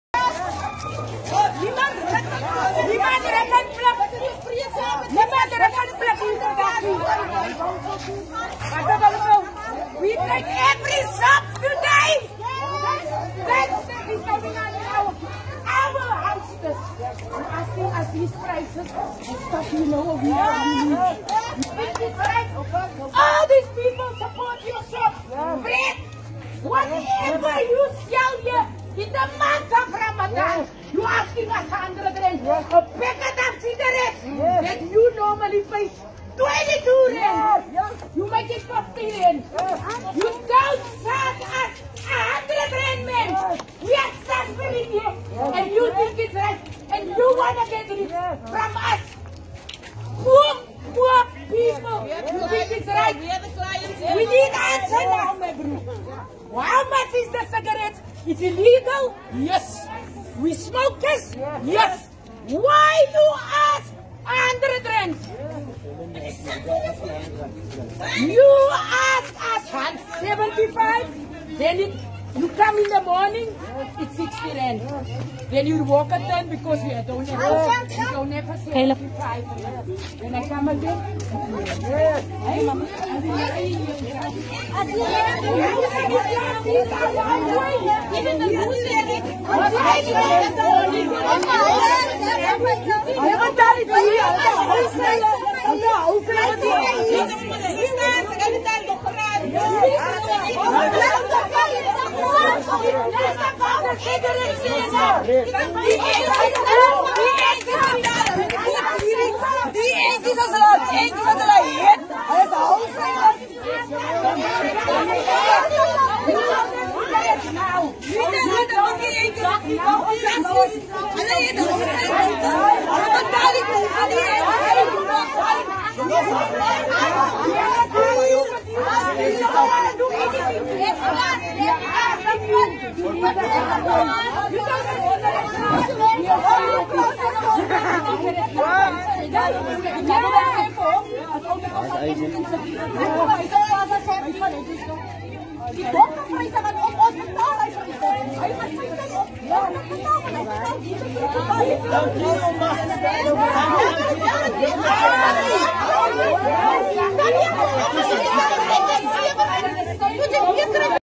Cigarette price increase are making people upset | South Africa
Cigarette price increase are making people upset. R100 a packet is what some people are selling it for